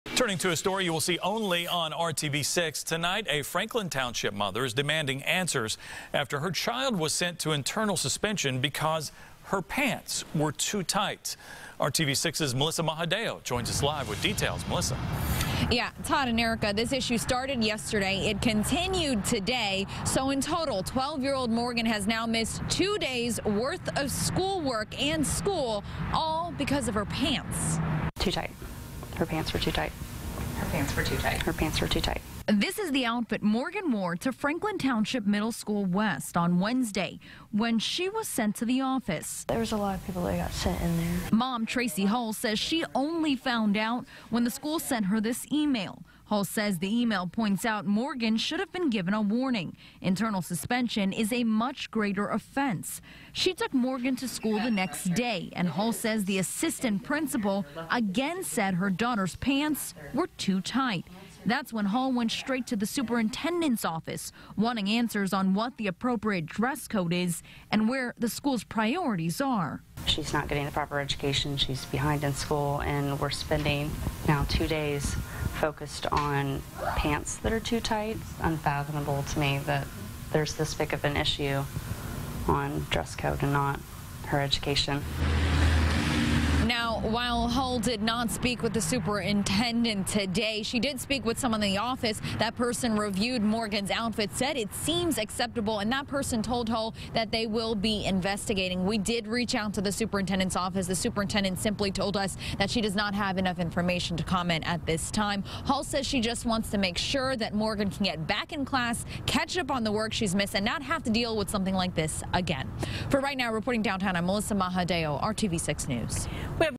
12-year-old suspended over pants
Franklin Township mother says punishment was too severe